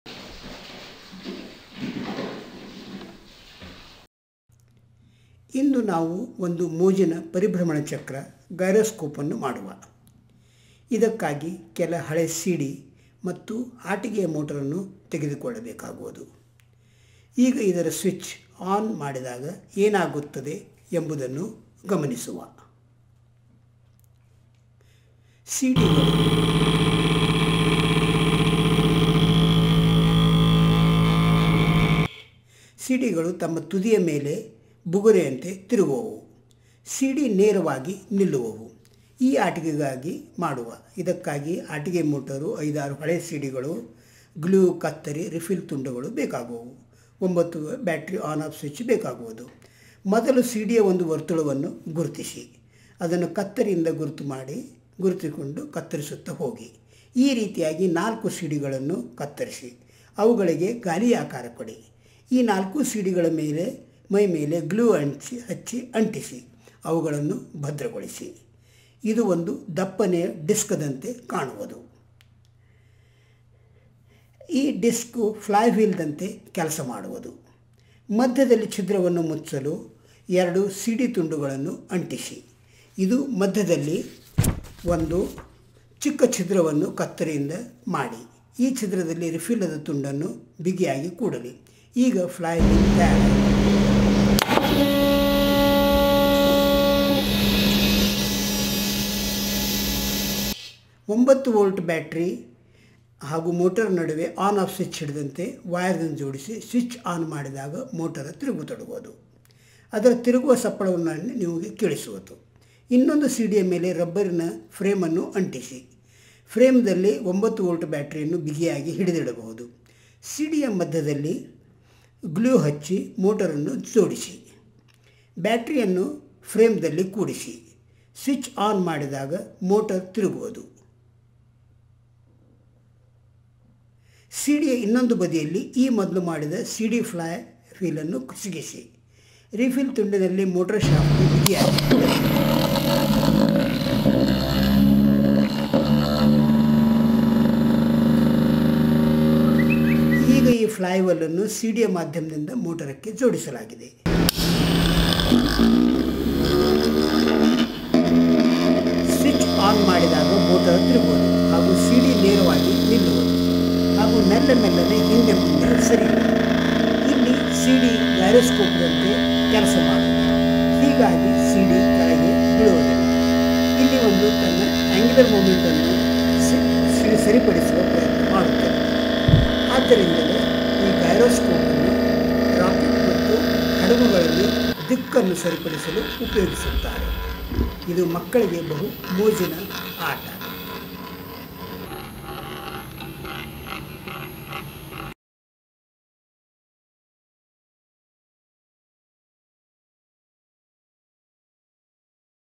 Simple CD Gyroscope | Kannada
As a heavy disc spins at a fast speed it prevents the big CD from falling. The CD GYROSCOPE stands erect and slowly spins on its point of contact. You will need 5 old CDs, Glue, scissors, 9-volt battery, switch, 9-volt toy motor etc. First mark a small circle on 4 old CDs and cut them with a scissors. Then glue these all four small CDs together to make a thick heavy disc. This will act like a flywheel. Stick two small pieces of CD on both sides to cover the big hole in the flywheel. Now drill a small hole in the center of the flywheel. Tight fit a piece of small ball pen refill in this hole.
Attach a 9-volt toy motor in series to a 9-volt battery and an ON/OFF switch. On switching ON the motor will spin. You can perhaps only hear the sound but not see the spinning motor shaft. Stick a rubber frame to an old CD. The frame will house the 9-volt battery. Fix the motor in the center of the CD with strong glue. Place the battery in its housing. Tie the switch. Then tight fit the FLYWHEEL on the motor shaft. On switching the motor the flywheel will spin very fast. Now stand the CD on a table. The CD will not fall down because of the spinning flywheel.
Why does the CD stand and not fall? The spinning flywheel acts like a GYROSCOPE. Its axis of rotation is free to assume any orientation by itself. Even if you tilt the CD it returns to its original position because of Conservation of Angular Momentum. That is why GYROSCOPES are used for maintaining orientation in Rockets and Ships.